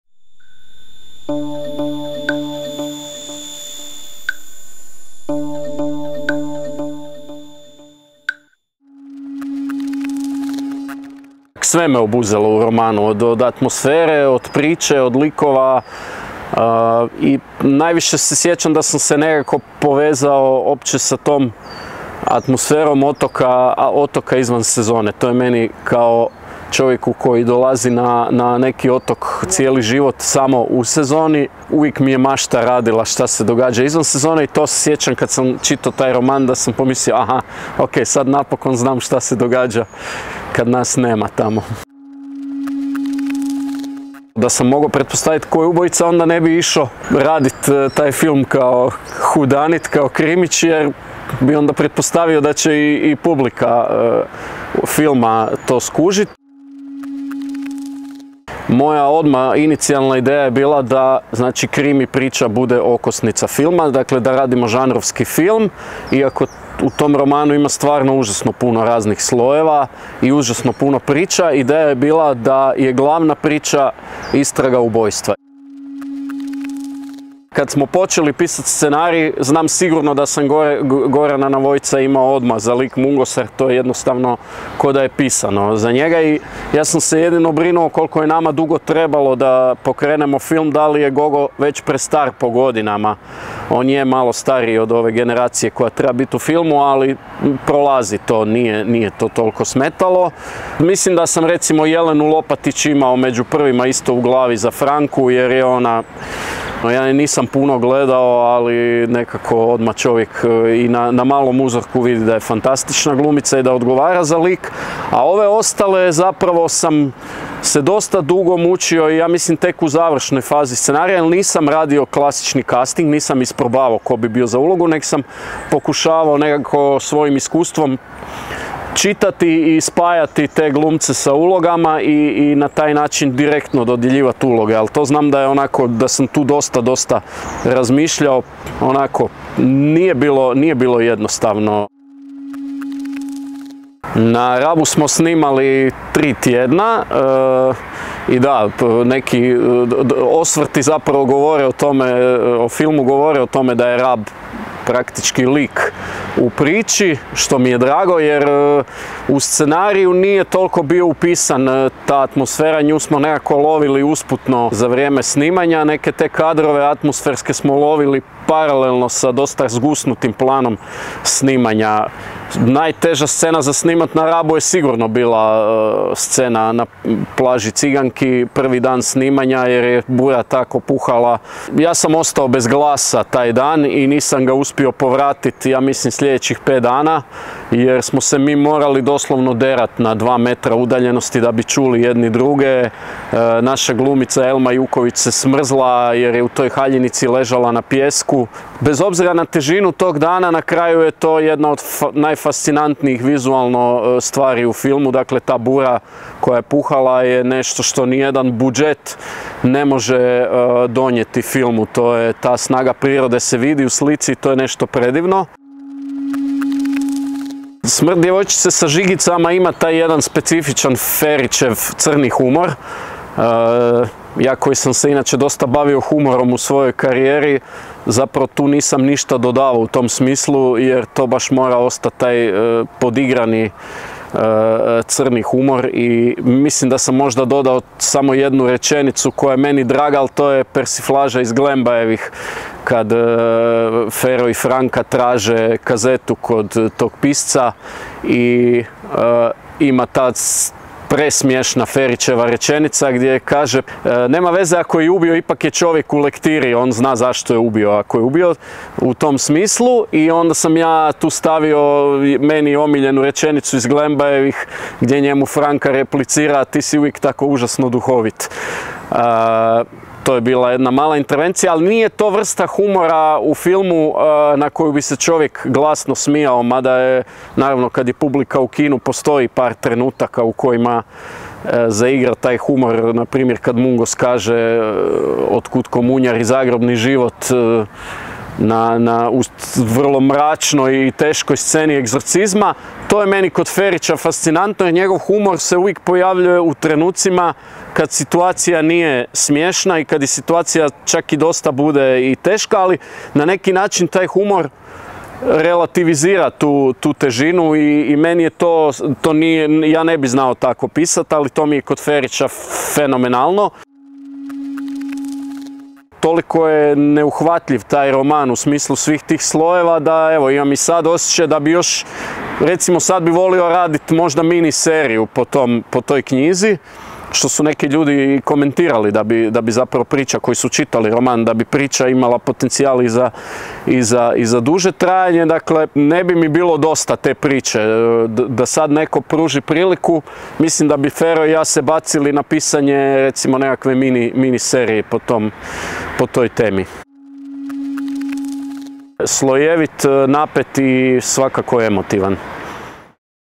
Otkrij Smrt djevojčice sa žigicama uz Gorana Kulenovića
U sklopu kampanje Hrvatskog audiovizualnog centra Otkrij naš novi film započete 2022., pokrećemo serijal kratkih video intervjua kojima nastojimo publici približiti filmske stvaraoce iza i ispred kamere, a čiji se radovi ove jeseni i zime prikazuju u hrvatskim kinima.

Prvi sugovornik je Goran Kulenović, redatelj i (ko)scenarist kriminalističkog trilera Smrt djevojčice sa žigicama.

Kamera & montaža: Sanja Bistričić Srića
Animacija: Sara Salamon
Vizualni identitet: Studio Šesnić & Turković